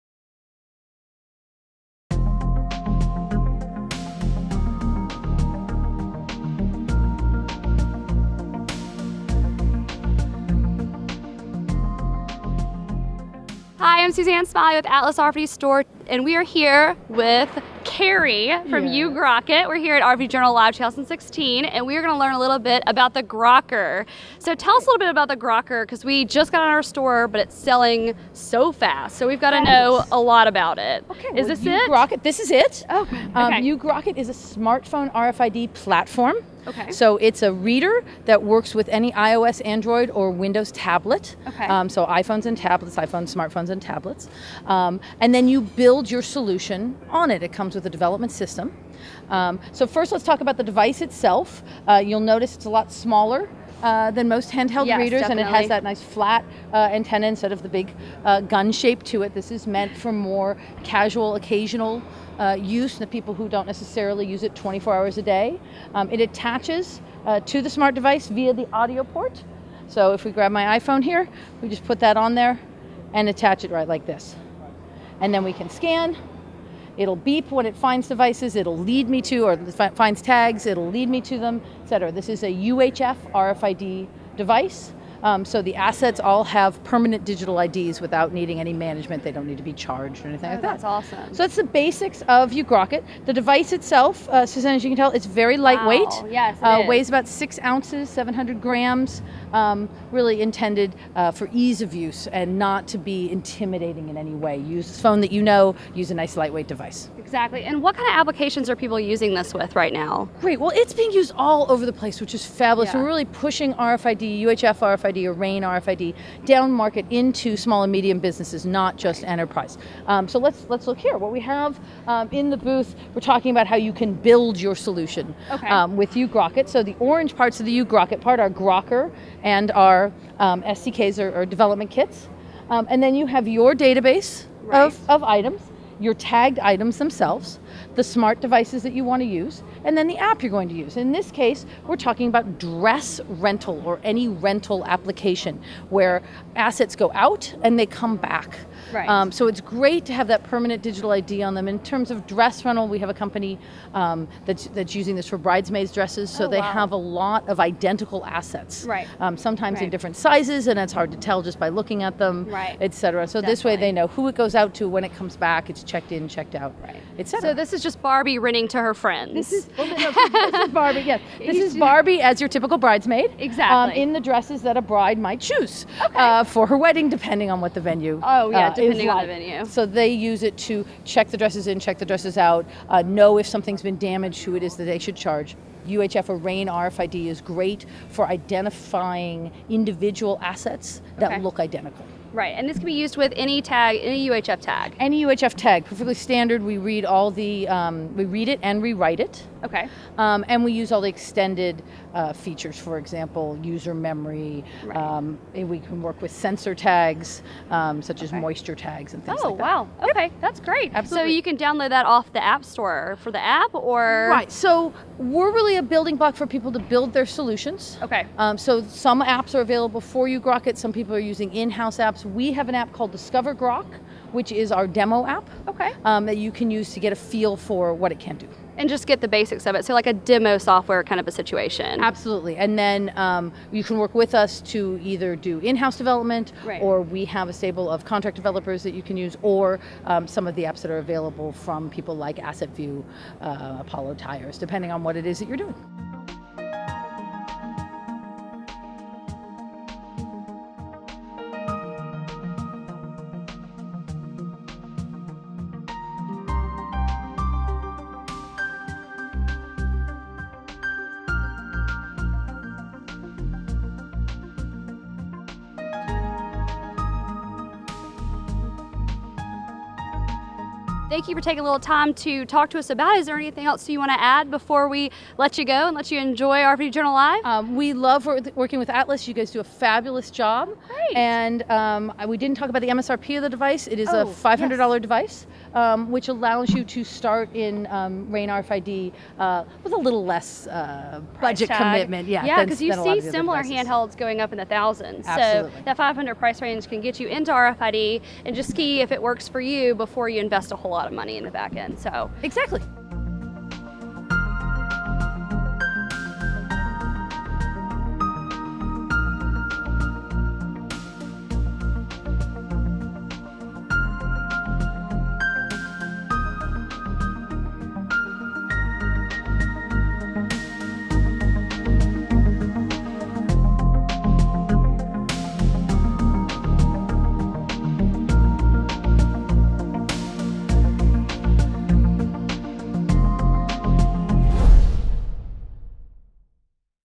U Grok It Booth Interview | RFID Journal Live! 2016
The U Grok It Grokker UHF RFID Reader is an easy-to-use, handheld RFID reader. It is a plug-and-play device that allows users to get started with RFID without the large costs typically associated with RFID readers. It is suited for most applications that are using RFID to identify, locate, track and take accurate inventory. With three unique apps on the Apple and Android app store, this reader contains everything you need to get started.